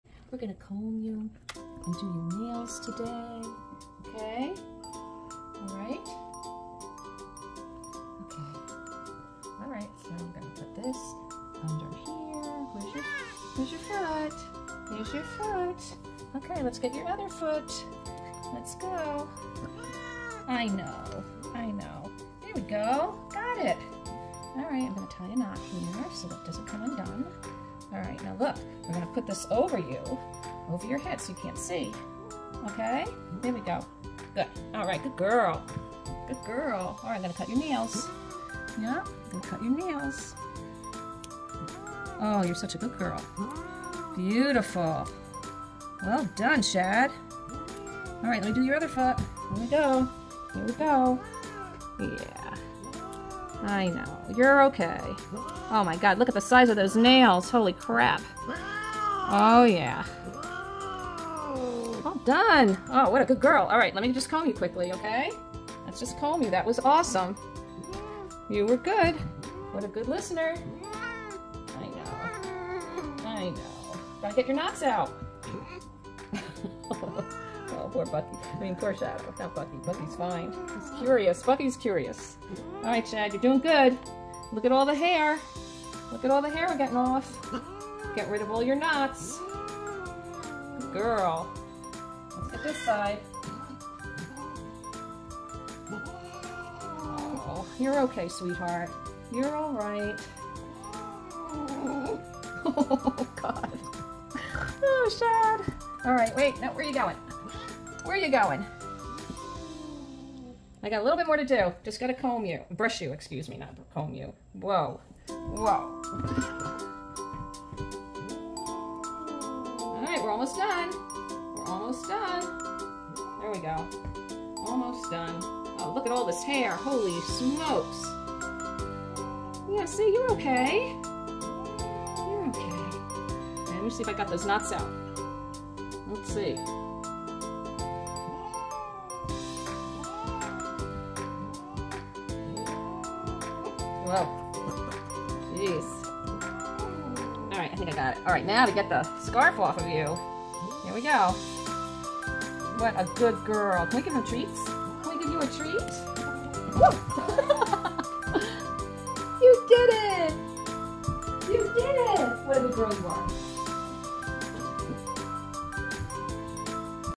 Shadow gets groomed...YIKES the DRAMA!!
Shadow is my loving 15 year old Siberian Forest cat who does not like getting her nails clipped.  She also will express her annoyance if she is relaxing on my lap and I want to comb her. She will try to scratch or bite me when I least expect it!   Soooo, grooming her to trim nails and get out her knots is a completely different experience!
I have to secure her feet so she doesn't scratch me and I cover her face as well!  Using a scarf has worked for years! She does get mad at me but it only lasts for about 5 minutes and she's back on my lap purring!
I often wondered if Shadow has been traumatized by a grooming experience as a kitten.  In the summer, previous owners used to take her to a grooming shop to have her back end shaved.  Hmmm